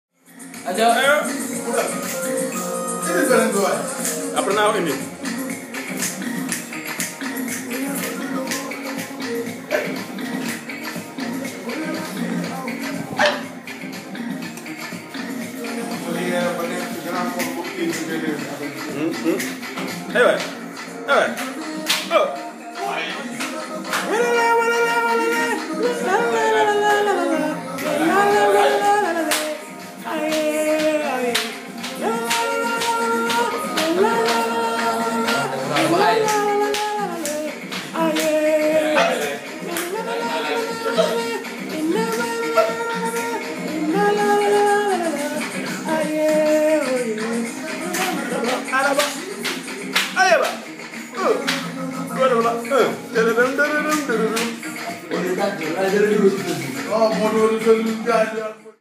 Quand le vigile se met au coupé decallé ca donne ça!
Quelque instants de délire pour déstresser un peu ...